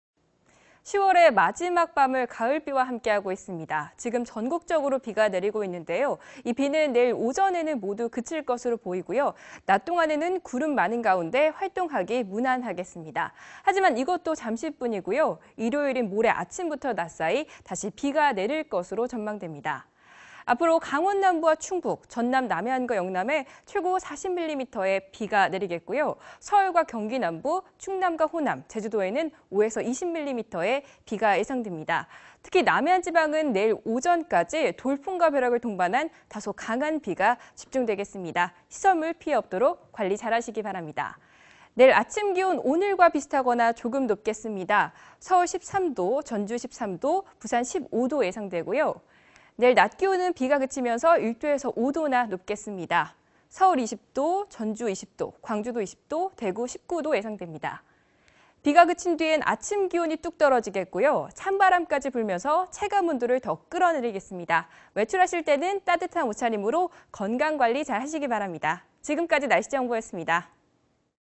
비 내일 오전에 대부분 그쳐… 서울 13 / 20℃ (2014-10-31) / YTN 웨더
10월의 마무리, 전국에 가을비와 함께하고 있습니다.이번 비 일요일까지 내렸다 그치기를 반복하겠는데요, 일단 내일 오전에 비가 대부분 그치겠고요. 이후에는 바깥 활동하기 무난하겠습니다. 하지만 일요일 아침부터 낮 사이 또 한 차례 비가 내리겠습니다.앞으로 내리는 비의 양은 강원 남부와 충북, 영남과 전남 남해안...